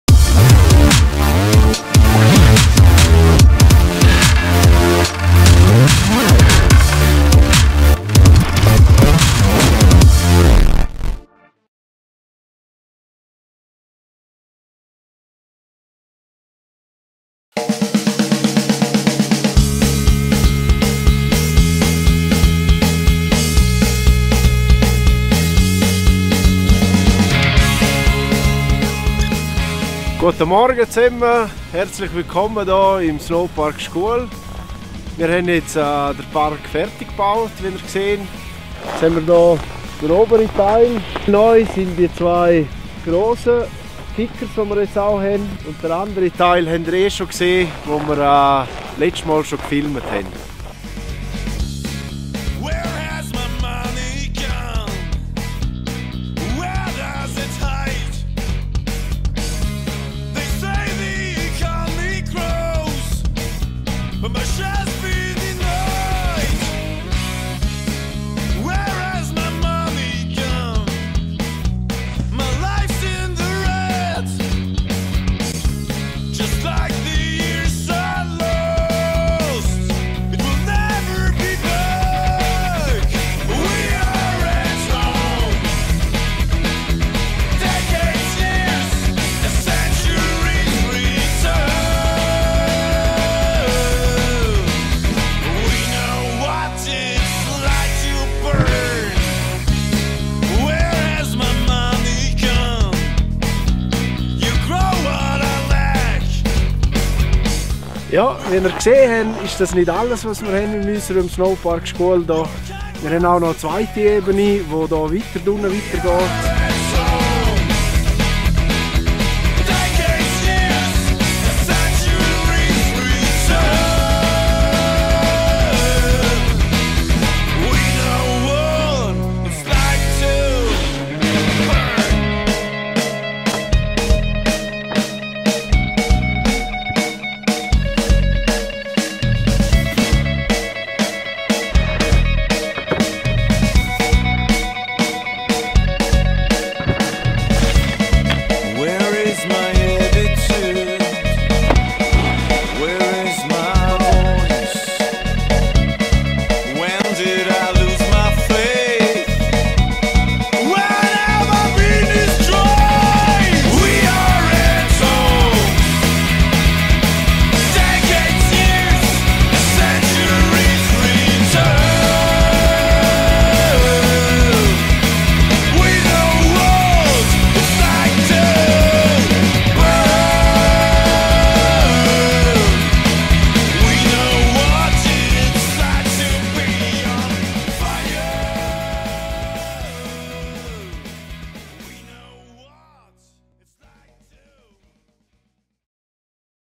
Scuol_11-02-2012_SNB_Easy Park Laps
Parkdesigner Curdin Erni makes freestyle dreams come true! A bunch of local snowboarders took the chance to be part of a film- and photoshooting which happened on saturday the 11th of february 2012! Enjoy the clip and make sure to visit the Snowpark Scuol as soon as possible! snowpark-scuol.com